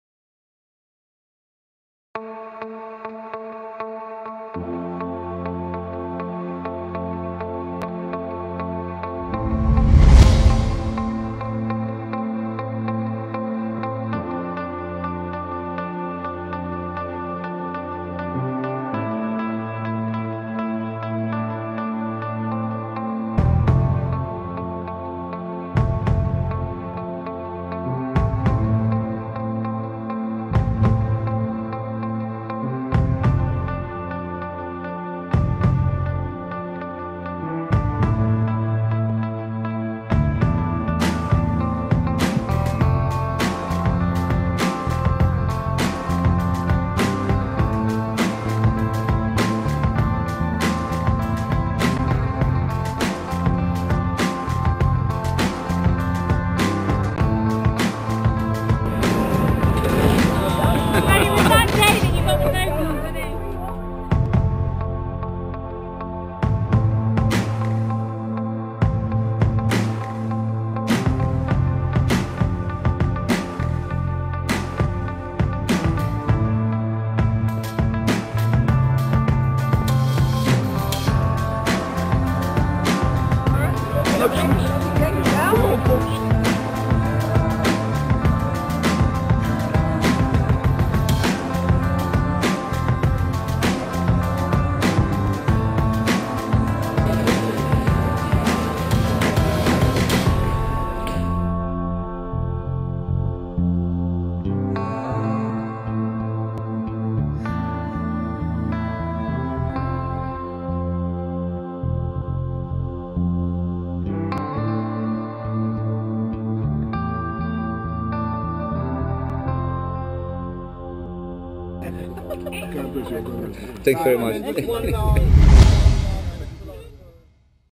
In the rings with Muhammad Ali and his brother Rahaman Ali
Korte impressie van 4 dagen Londen tijdens het outreache program van Muhammad Ali op uitnodiging van Everlast.

H.Veenendaal